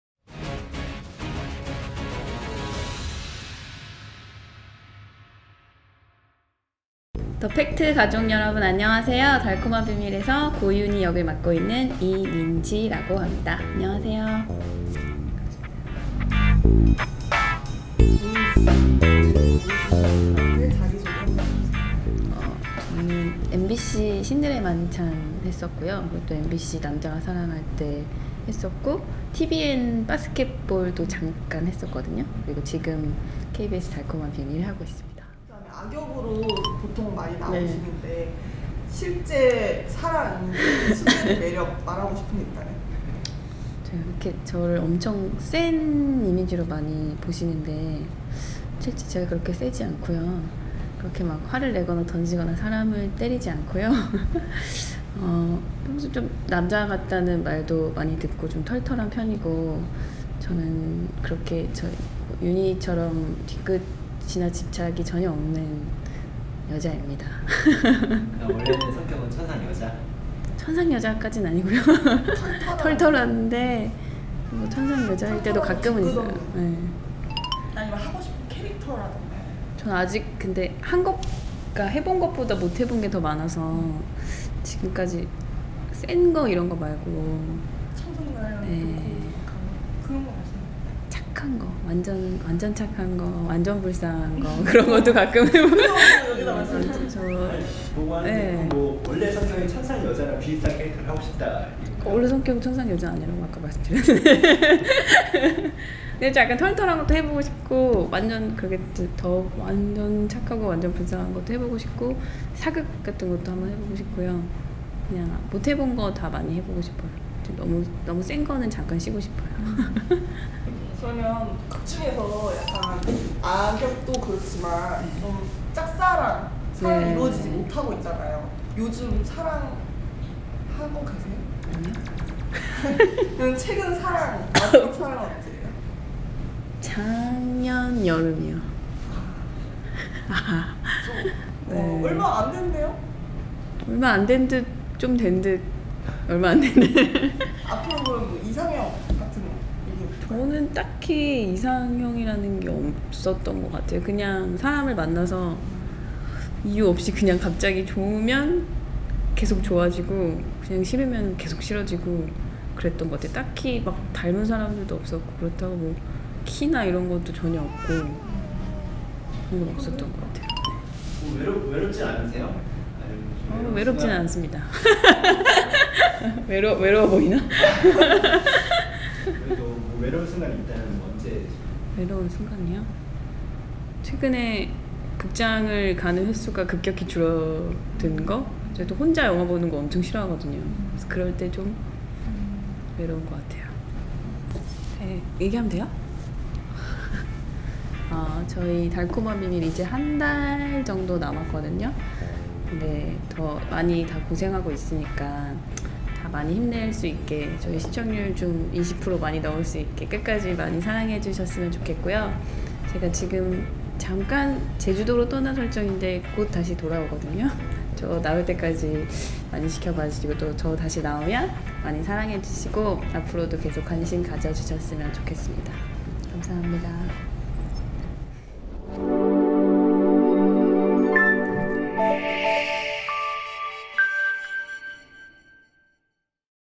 [TF영상] '달콤한 비밀' 이민지, '악역 이미지? 변신하고 싶어'
KBS2 일일드라마 '달콤한 비밀'의 배우 이민지가 지난달 24일 서울 금천구 가산동 더팩트를 찾았다.
이민지는 '달콤한 비밀'에서 악역으로 등장해 시청자에게 기분 좋은 미움을 받고 있지만, 이날 인터뷰를 통해 악역과는 다른 반전 매력을 보였다. 인터뷰 내내 차가운 이미지와는 다른 순수한 미소를 연신 보이며 애교도 서슴지 않았다.
인터뷰 중 '연애'에 대한 질문을 하자 이민지는 "작년 여름이 마지막 사랑이에요. 영화관 가는 횟수가 점점 줄어드네요. 이상형은 따로 없어요~"라며 애써 외로움을 호탕한 웃음으로 넘겼다.